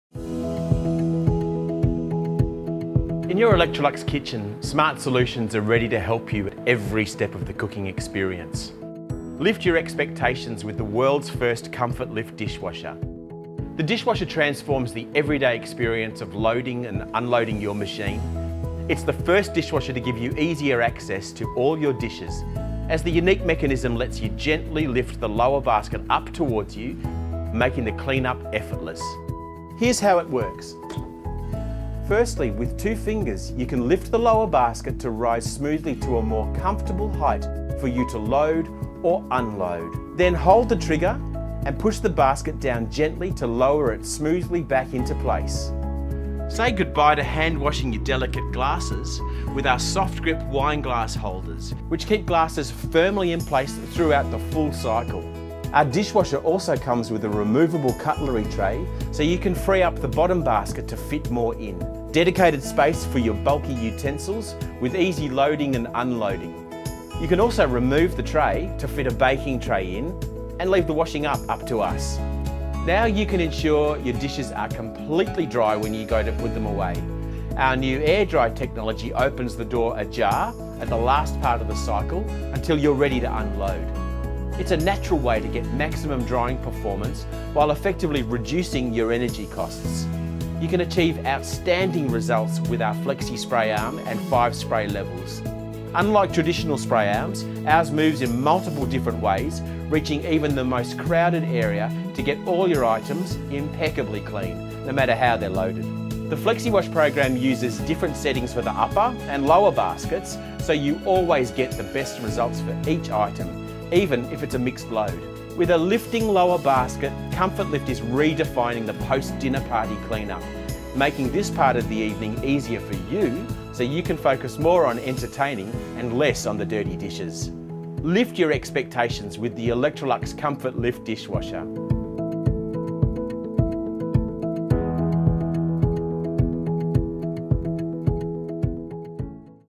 Electrolux ESF8735ROX 60cm Dishwasher with ComfortLift @ JB HIFI
60cm built-under dishwasher with ComfortLift™, AirDry technology, light beam on floor indicator and 14 place settings.